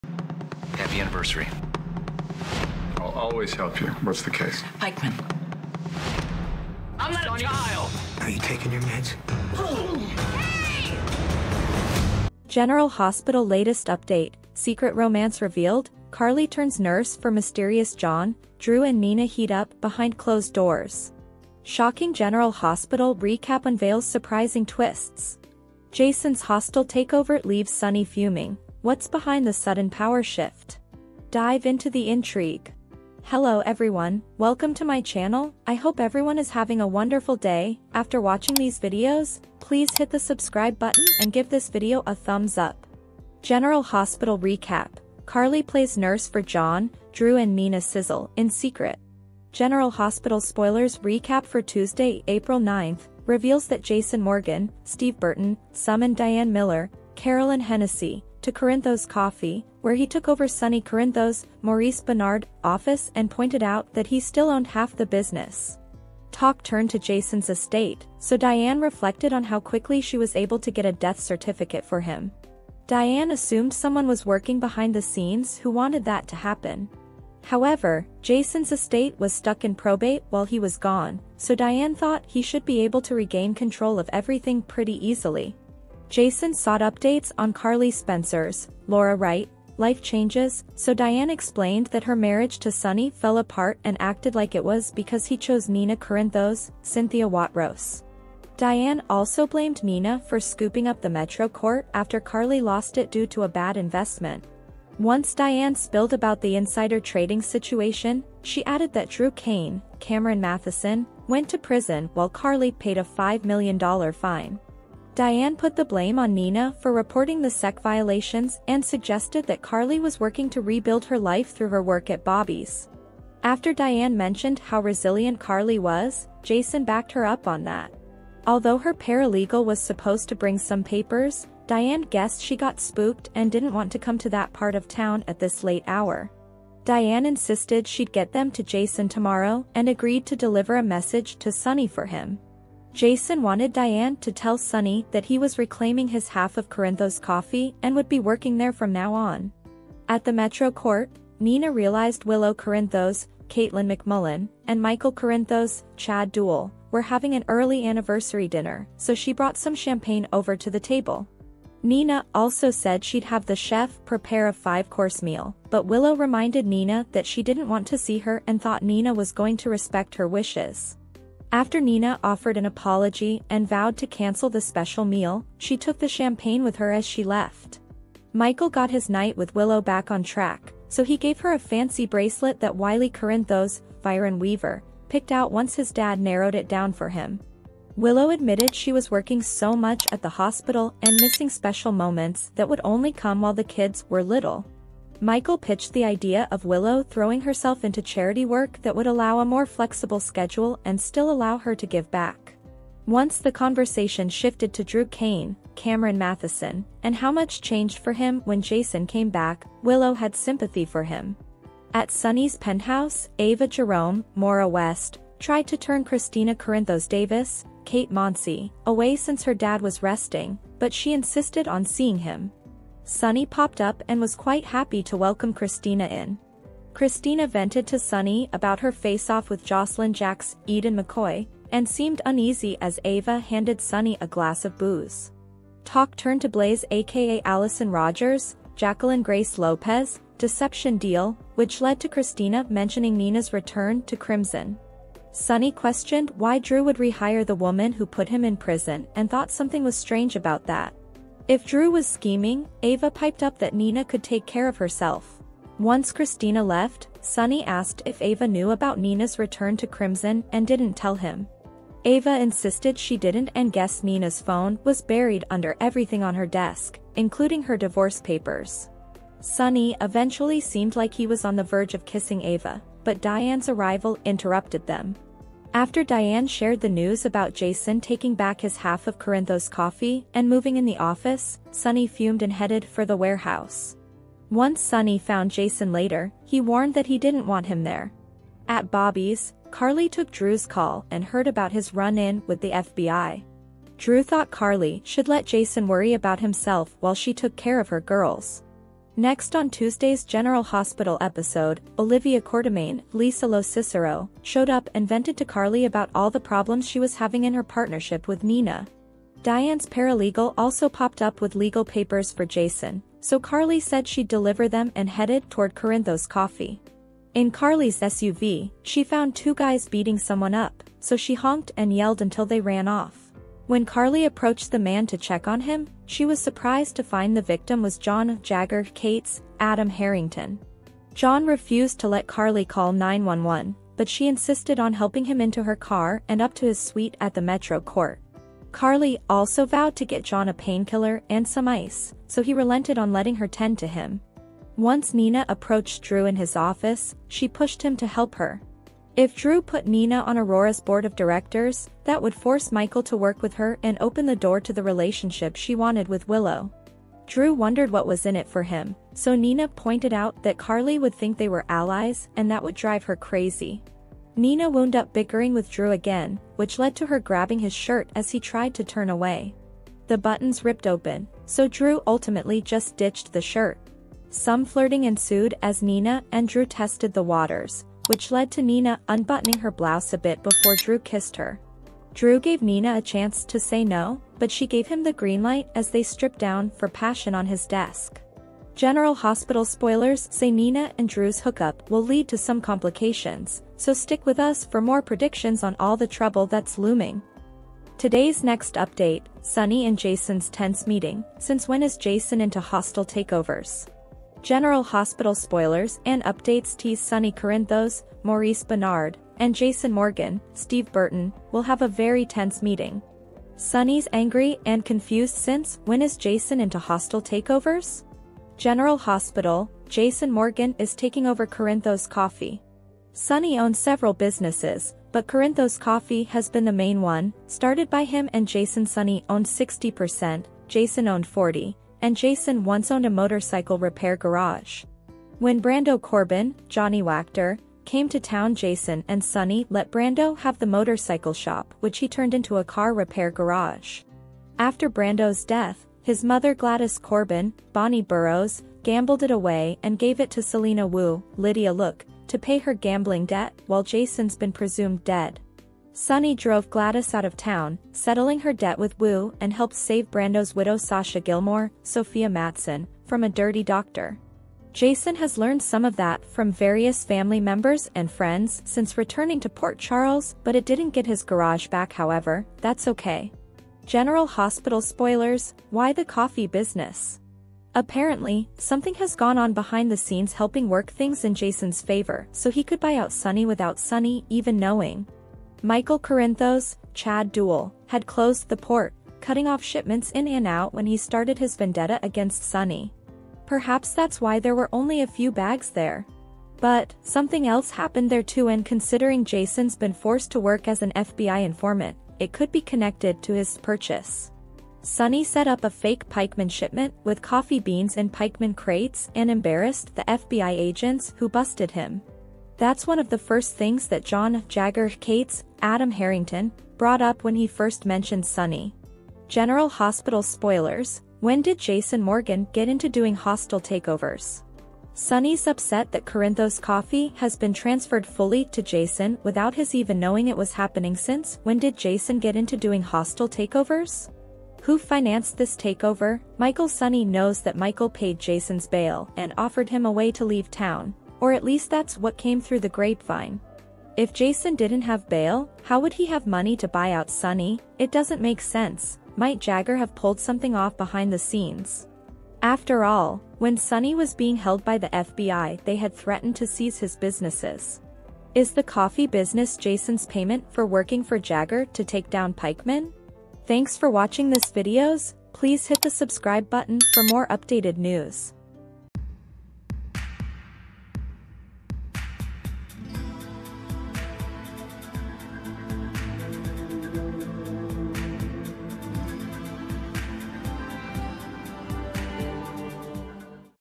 Sonny vs. Jason! Alert: Carly's Surprising Role as Nurse to John Revealed! Nina's Steamy Affair!
General hospital
General hospital update news